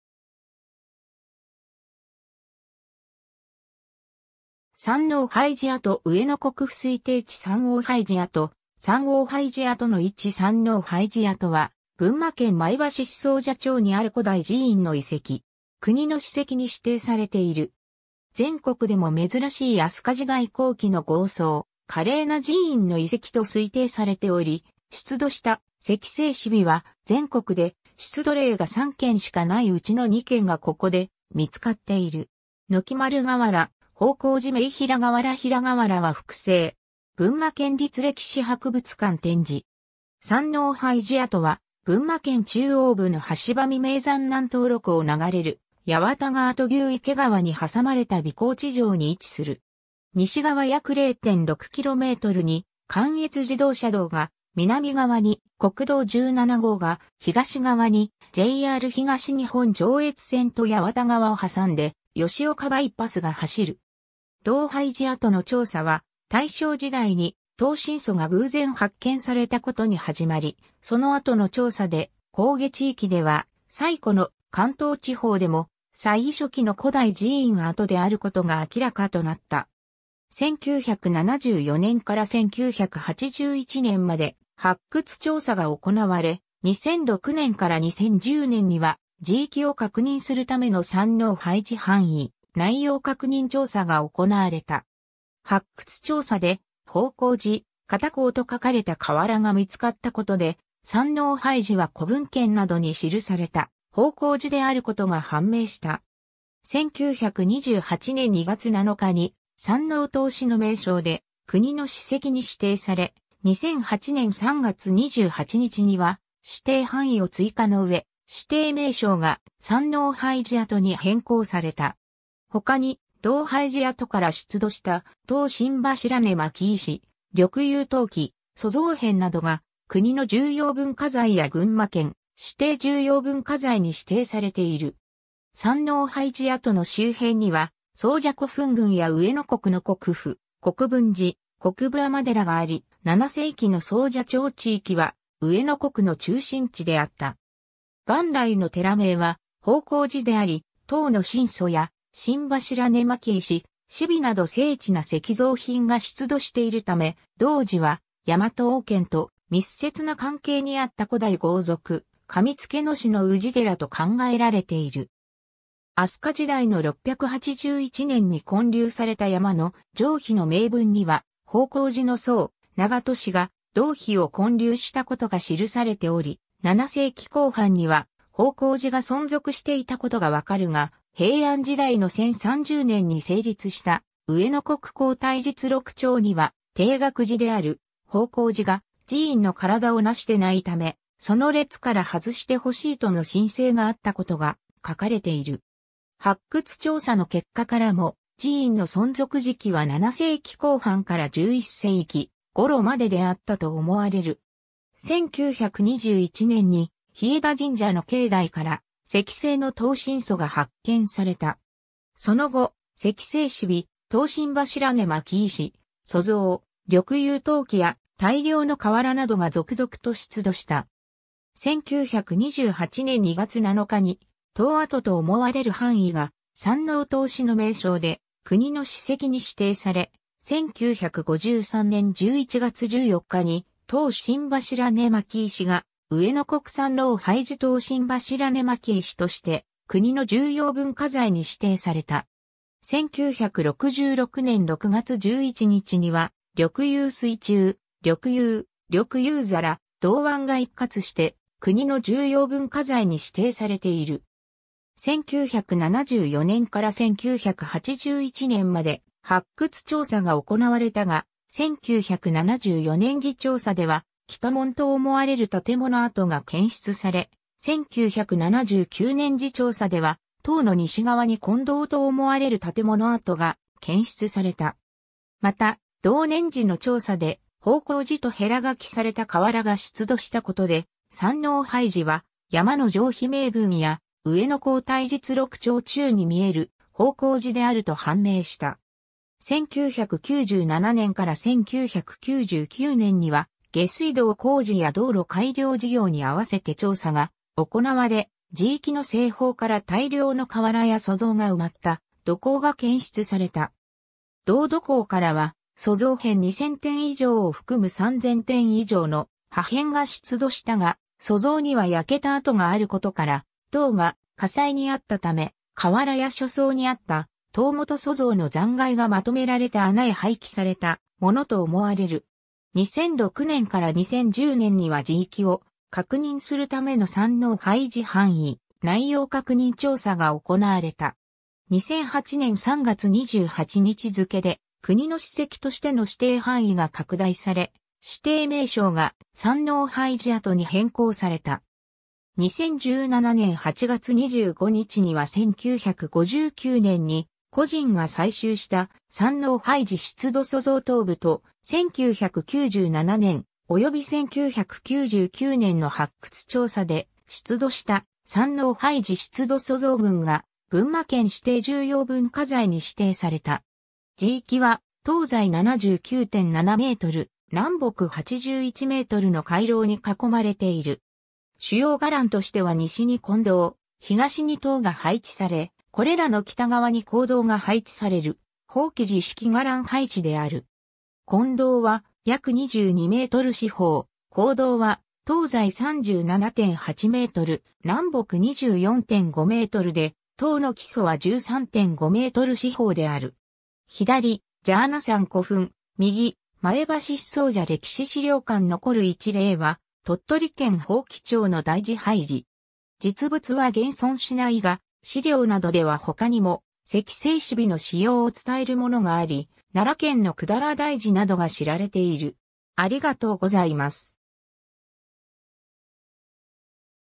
山王廃寺跡
山王廃寺跡 上野国府 推定地 山王 廃寺跡 山王廃寺跡の位置 山王廃寺跡（さんのうはいじあと）は、群馬県前橋市総社町にある古代寺院の遺跡。
全国でも珍しい飛鳥時代後期（白鳳期）の豪壮・華麗な寺院の遺跡と推定されており、出土した石製鴟尾は全国で出土例が3件しかないうちの2件がここで見つかっている。
 軒丸瓦・「放光寺」銘平瓦平瓦は複製。
群馬県立歴史博物館展示。
 山王廃寺跡は、群馬県中央部の榛名山南東麓を流れる八幡川と牛池川に挟まれた微高地上に位置する。
西側約0.6kmに関越自動車道が、南側に国道17号が、東側にJR東日本上越線と八幡川を挟んで吉岡バイパス（産業道路）が走る。
同廃寺跡の調査は大正時代に塔心礎が偶然発見されたことに始まり、その後の調査で、上毛地域では最古の、関東地方でも最初期の古代寺院跡であることが明らかとなった。
 1974年（昭和49年）から1981年（昭和56年）まで発掘調査が行われ、2006年（平成18年）から2010年（平成22年）には寺域を確認するための山王廃寺範囲内容確認調査が行われた。
発掘調査で「放光寺」・「方光」と書かれた瓦が見つかったことで、山王廃寺は古文献などに記された放光寺であることが判明した。
 1928年（昭和3年）2月7日に「山王塔阯」の名称で国の史跡に指定され、2008年（平成20年）3月28日には指定範囲を追加のうえ、指定名称が「山王廃寺跡」に変更された。
他に、同廃寺跡から出土した塔心柱根巻石、緑釉陶器、塑像片などが国の重要文化財や群馬県指定重要文化財に指定されている。
 山王廃寺跡の周辺には総社古墳群や上野国の国府・国分寺・国分尼寺があり、7世紀の総社町地域は上野国の中心地であった。
元来の寺名は「放光寺」であり、塔の心礎や心柱根巻石、鴟尾など精緻な石造品が出土しているため、同寺はヤマト王権と密接な関係にあった古代豪族・上毛野氏の氏寺と考えられている。
 飛鳥時代の681年（天武天皇10年）に建立された山ノ上碑の銘文には、放光寺の僧・長利が同碑を建立したことが記されており、7世紀後半には放光寺が存続していたことがわかるが、平安時代の1030年（長元3年）に成立した『上野国交替実録帳』には、定額寺である放光寺が寺院の体をなしてないため、その列から外してほしいとの申請があったことが書かれている。
発掘調査の結果からも、寺院の存続時期は7世紀後半から11世紀頃までであったと思われる。
 1921年（大正10年）に、日枝神社の境内から石製の塔心礎が発見された。
その後、石製鴟尾、塔心柱根巻石、塑像、緑釉陶器や大量の瓦などが続々と出土した。
 1928年（昭和3年）2月7日に塔跡と思われる範囲が「山王塔阯」の名称で国の史跡に指定され、1953年（昭和28年）11月14日に塔心柱根巻石が「上野国山王廃寺塔心柱根巻石」として国の重要文化財に指定された。
1966年（昭和41年）6月11日には緑釉水注、緑釉埦、緑釉皿、銅鋺が一括して国の重要文化財に指定されている。
 1974年（昭和49年）から1981年（昭和56年）まで発掘調査が行われたが、1974年（昭和49年）次調査では北門と思われる建物跡（のちに講堂と推定）が検出され、1979年（昭和54年）次調査では塔の西側に金堂と思われる建物跡が検出された。
また、同年次の調査で「放光寺」とヘラ書きされた瓦が出土したことで、山王廃寺は山ノ上碑銘文や『上野交替実録帳』中に見える放光寺であると判明した。
 1997年（平成9年）から1999年（平成11年）には下水道工事や道路改良事業に合わせて調査が行われ、寺域の西方から大量の瓦や塑像が埋まった土坑が検出された。
同土坑からは塑像片2000点以上を含む3000点以上の破片が出土したが、塑像には焼けた跡があることから、塔が火災に遭ったため、瓦や初層にあった塔本塑像の残骸がまとめられて穴へ廃棄されたものと思われる。
 2006年（平成18年）から2010年（平成22年）には寺域を確認するための山王廃寺範囲内容確認調査が行われた。
2008年（平成20年）3月28日付けで国の史跡としての指定範囲が拡大され、指定名称が「山王廃寺跡」に変更された。
2017年（平成29年）8月25日には1959年（昭和34年）に個人が採集した「山王廃寺出土塑像頭部...